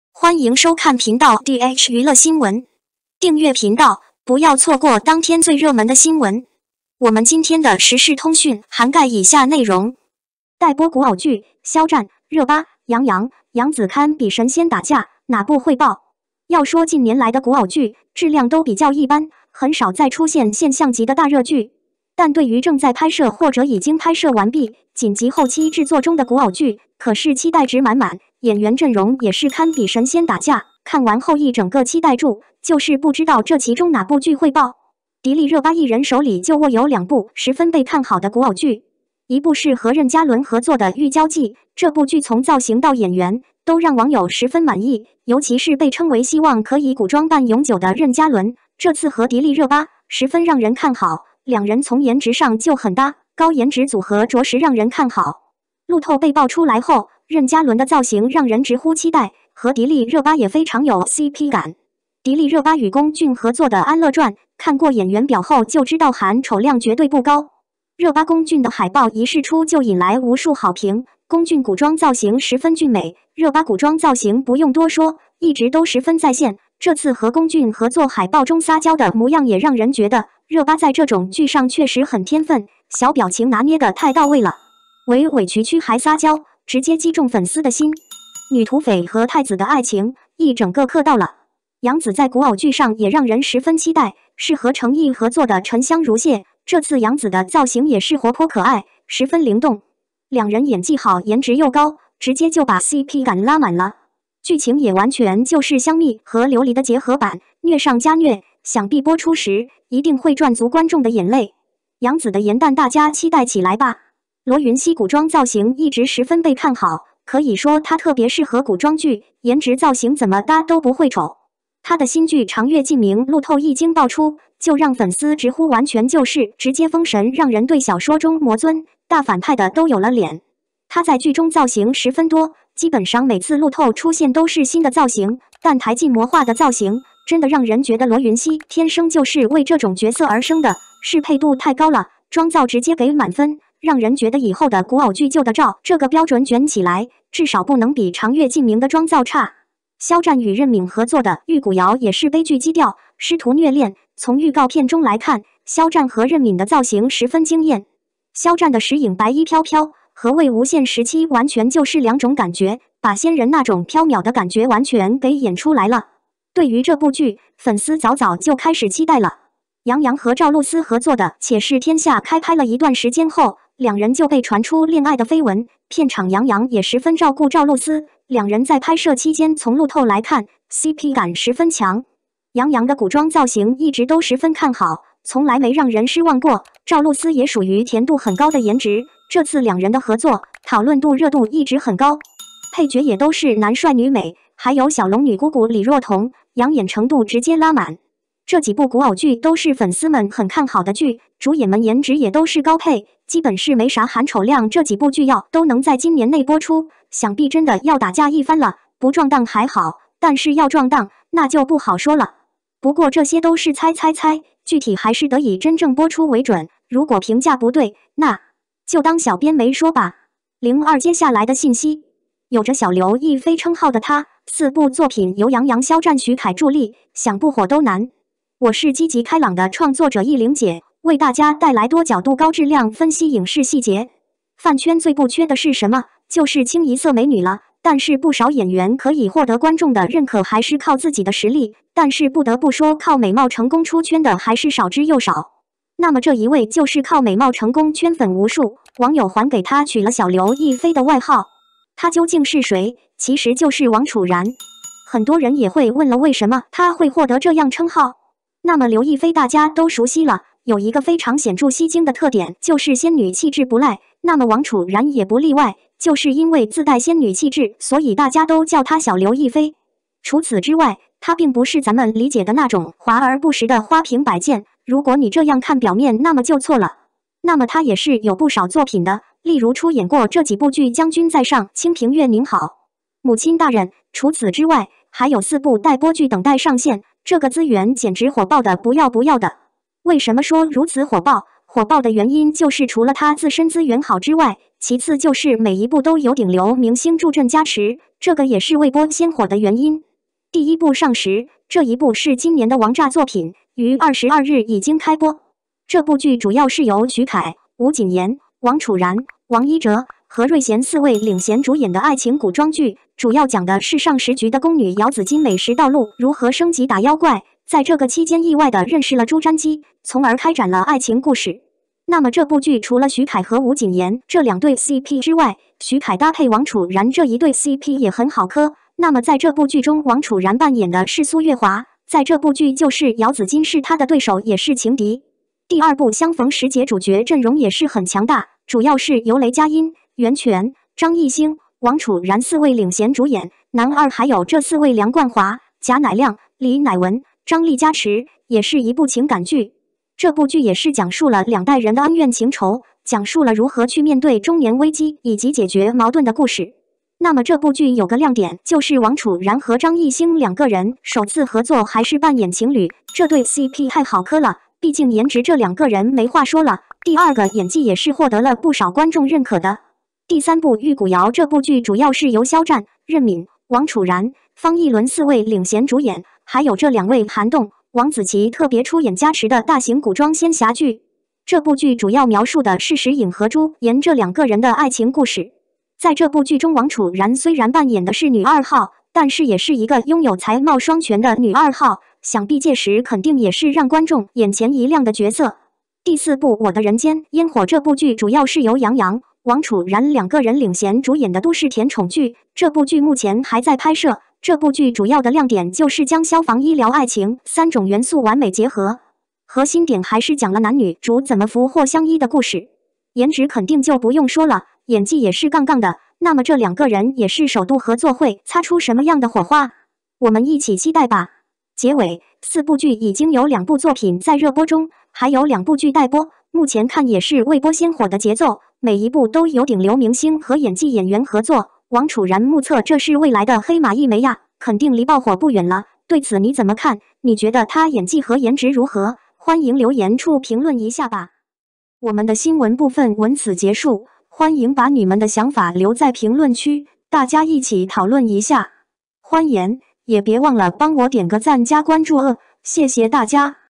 待播古偶剧，肖战、热巴、杨洋、杨紫堪比“神仙打架”，哪部会爆
00:10 待播古偶剧，肖战、热巴、杨洋、杨紫堪比“神仙打架”，哪部会爆
03:40 有着小刘亦菲称号的她，4部作品由杨洋肖战许凯助力，想不火都难
ENG SUB